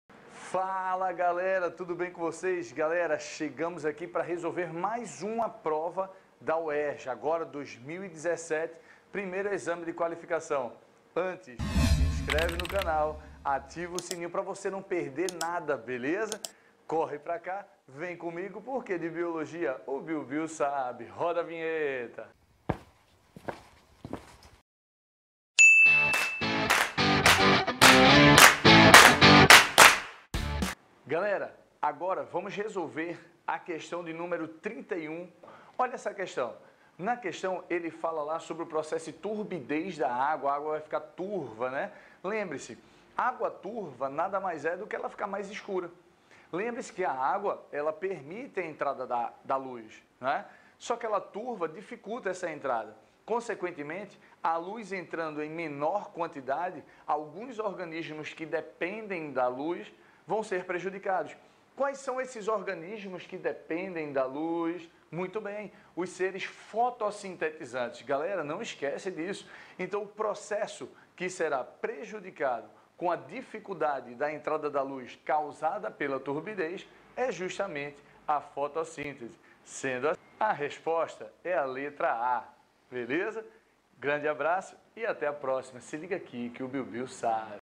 Resolução do exercício 31 - UERJ 2017 - 1º EQ [BIO BIO SABE]
Fala Alfredão ! Fala Jéssica ! Tudo bem ?

Nesse vídeo faço passo a passo da questão 31 do 1º Exame de Qualificação da UERJ 2017.
Prepare o lápis e o caderno e vem comigo !